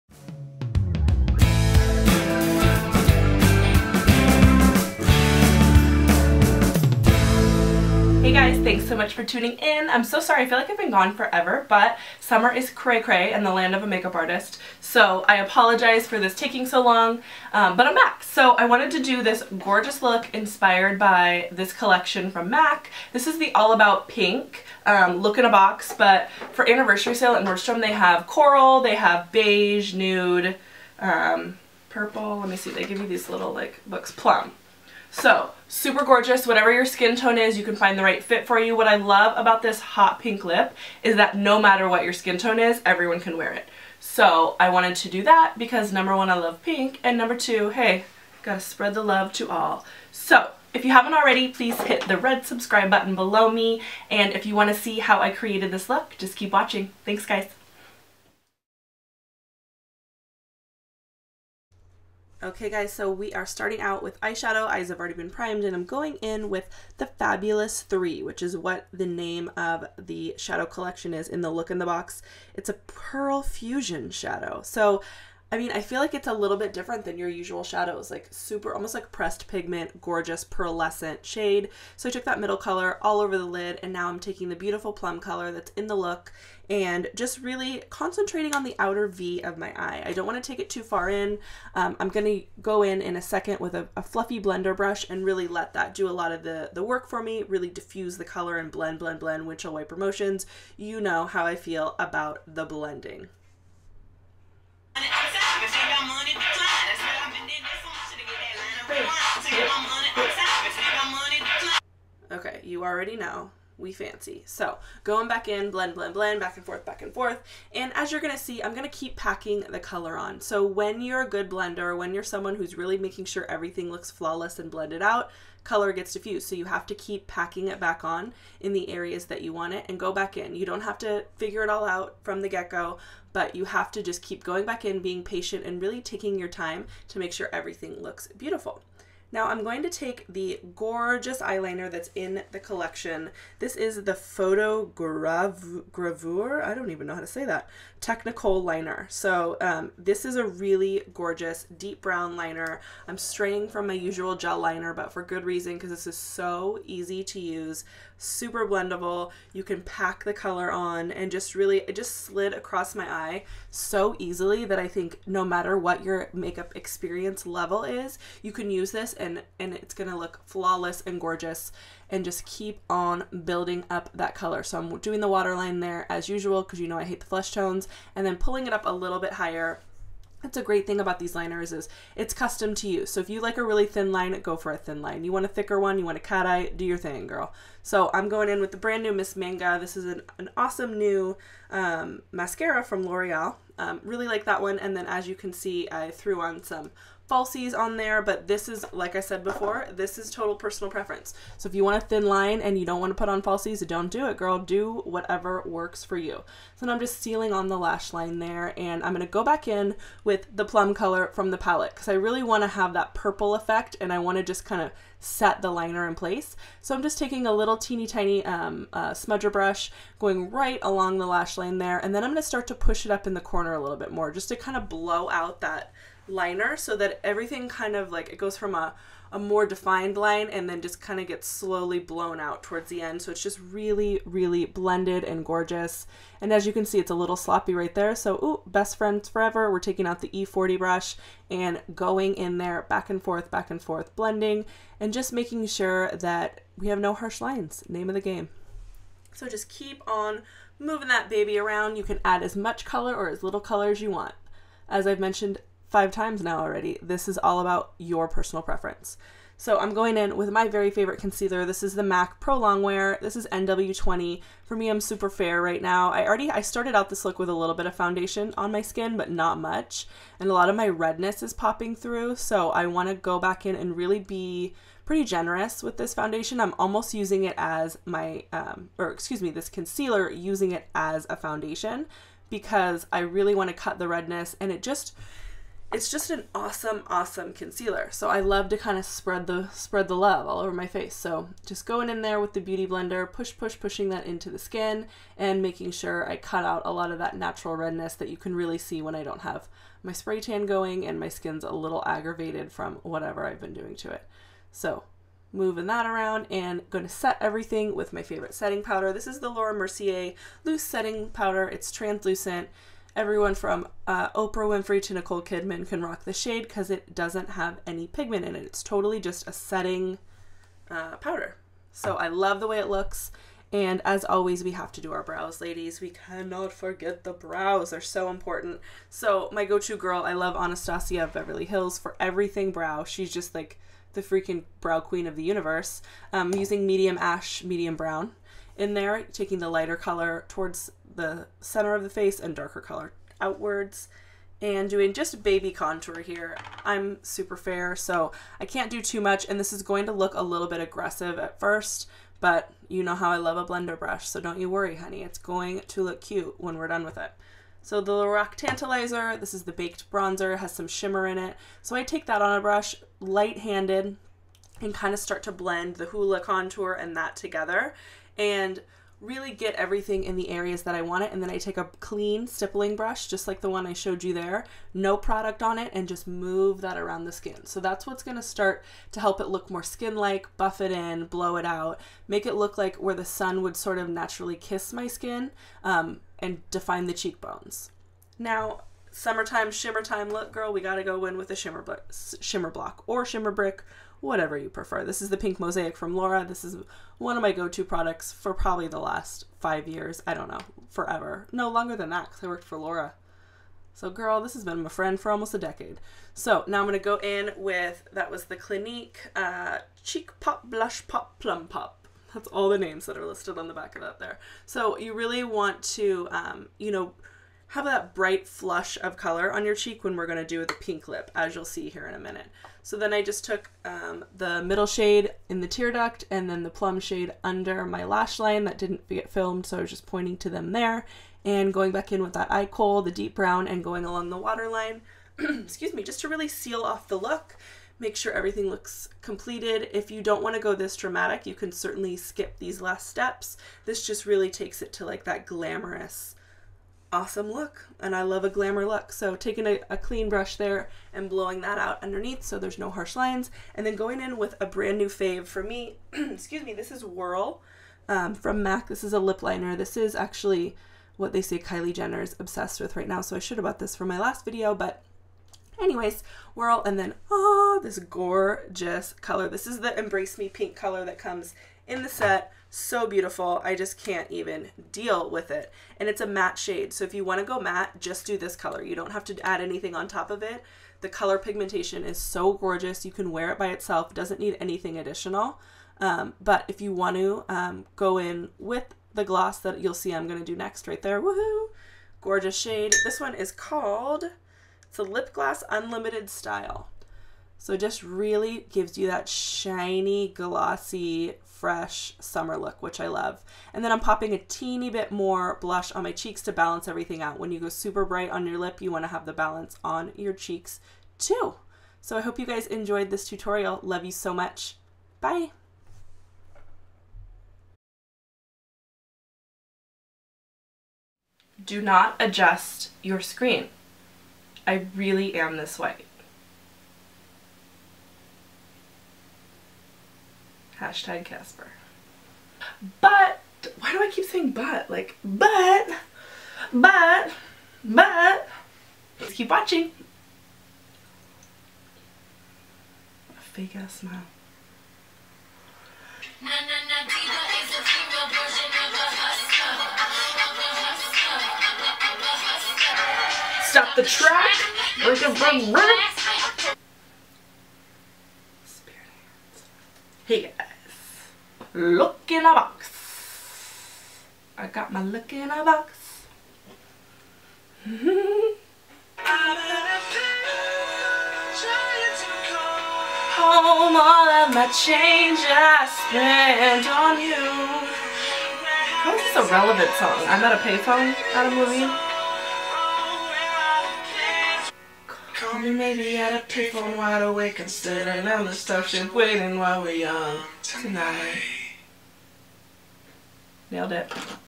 Bright Pink Lips | A Pop of Color Tutorial
All about PINK!  These hot pink lips will be the star of the show- It's a inspired by MAC's "Look in a Box".

Porducts Used-
M.A.C. Look in a Box- All About Pink (on sale during Nordstrom Anniversary).  This box includes the Fabulous Three Eyeshadow trio, Photogravure technakohl liner- kajal, Matte Embrace Me lipstick & Lipglass Unlimited Style gloss.
Revlon Color Stay Whipped foundation- 150 Buff
MAC Pro Longwear Concealer NW20
Laura Mericier Translucent Setting Powder
Benefit Hoola Bronzer
LORAC TANtilizer Baked Bronzer
Clinique Plum Pop Blush
Laura Mercier Shimmer Block in Pink Mosaic
Miss Manga Mascara by Loreal
Glamour Wispies by Ardell (false lashes)
MAC Whirl Lip Liner
Anastasia of Beverly Hills Brow Powder in Medium Ash/Brown